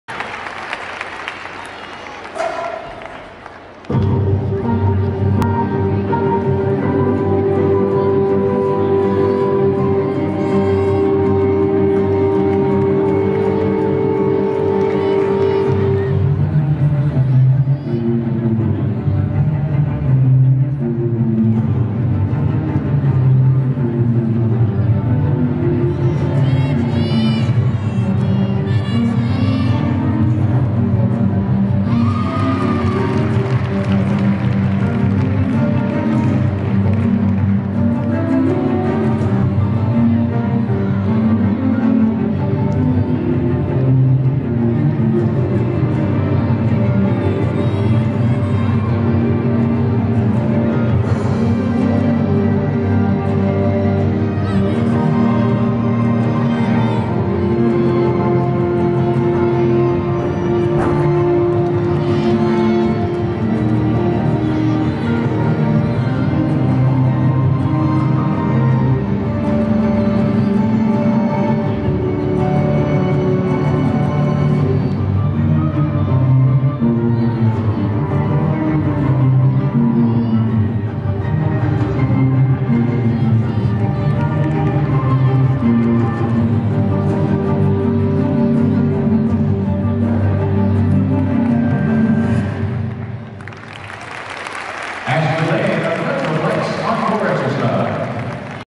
Ashley Lane - Floor Exercise - 2018 Nastia Liukin Cup
Score: 8.650
March 2, 2018 - Sears Centre Arena - Hoffman Estates, Ill.